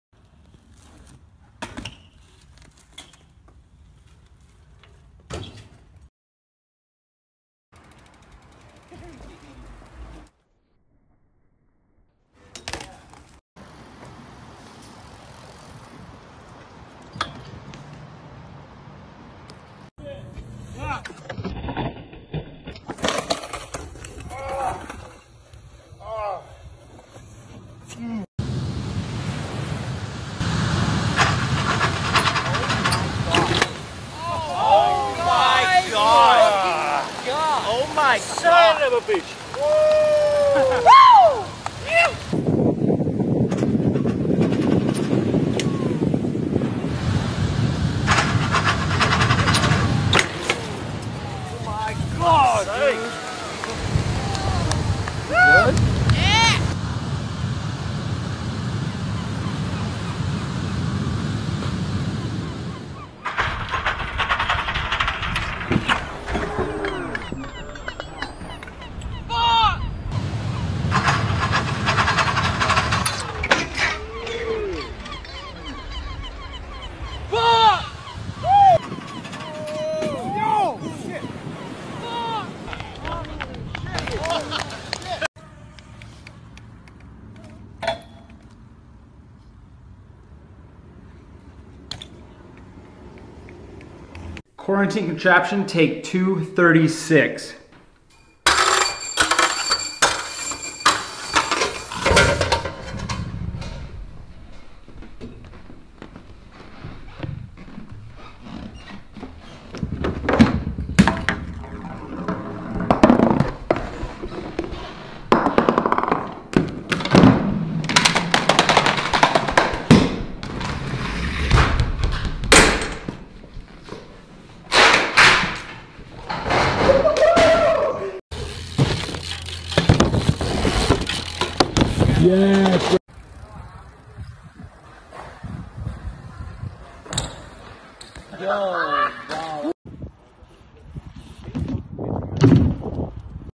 This Weeks Best BMX InstaCuts Street Clips #16 | Instagram 2020 Compilation
This weeks best IG Bmx street clips!  May 5 - May 12